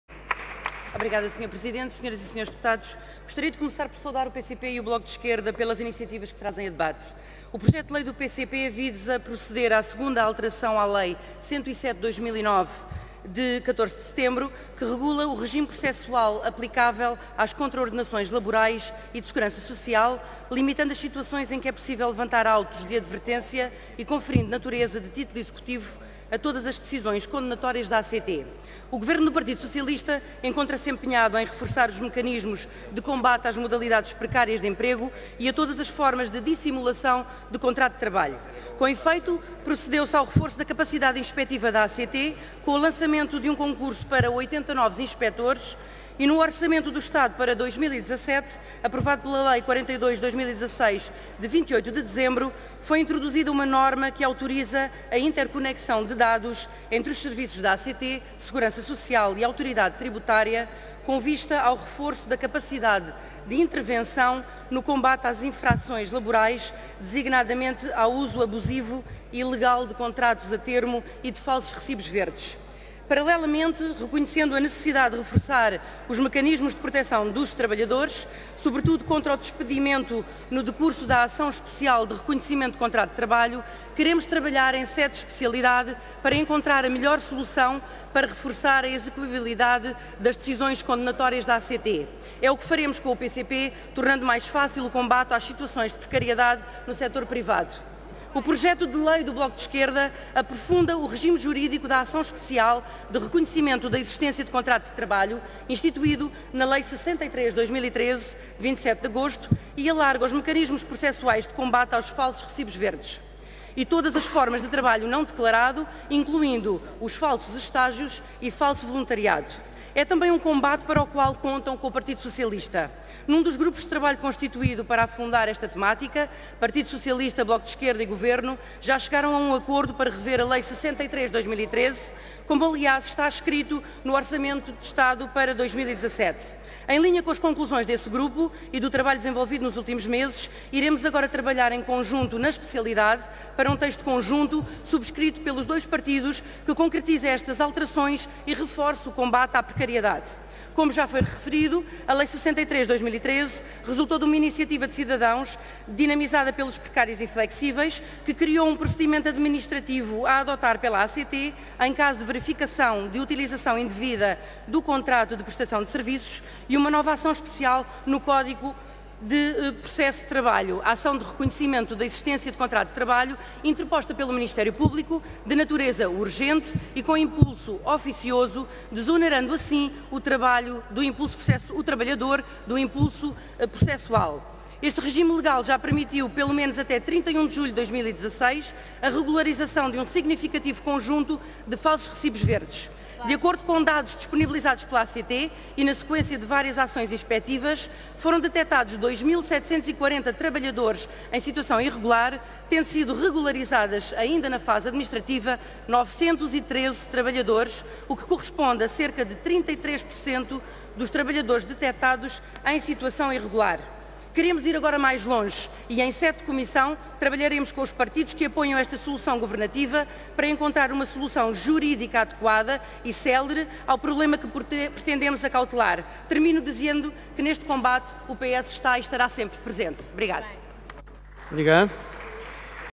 24-03-2017 - Debate Parlamentar | Autoridade para as Condições do Trabalho | Sofia Araújo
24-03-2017 | Intervenção do Deputado Sofia Araújo no âmbito dos Projectos de Lei (PCP, BE) para conferir natureza de título executivo às decisões condenatórias da ACT